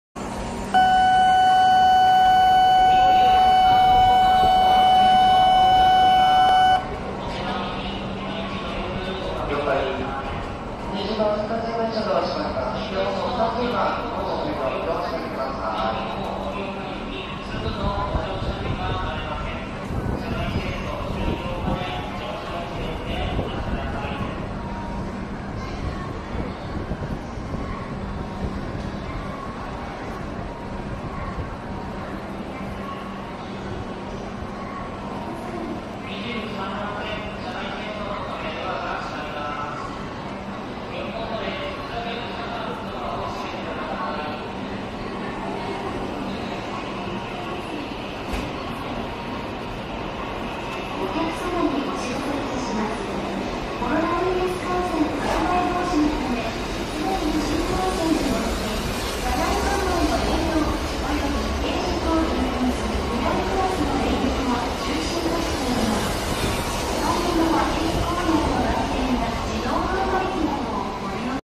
【北陸新幹線】W7系東京駅発車シーン
「1日1鉄」を目標に横浜をはじめとした全国の鉄道風景や旅行動画をお送りします

北陸新幹線W7系が東京駅を発車する映像です。

2021年6月撮影